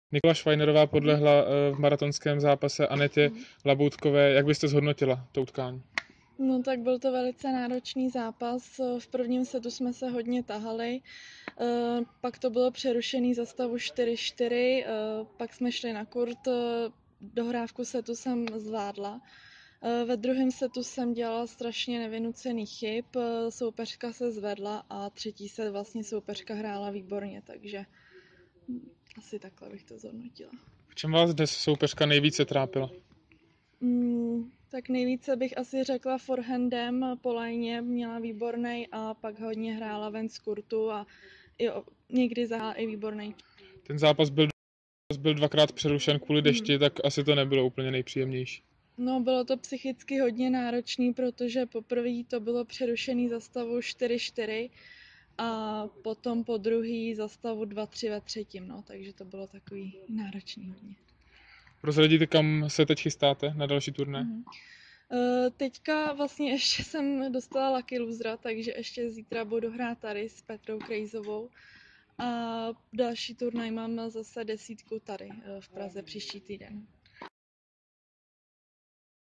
Nikola Schweinerova po prohře ve finále kvalifikace turnaje Kuchyně Gorenje Open 2016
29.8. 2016, finále kvalifikace turnaje Kuchyně Gorenje Prague Open 2016: Nikola Schweinerova - Aneta Laboutková 7:5, 2:6, 3:6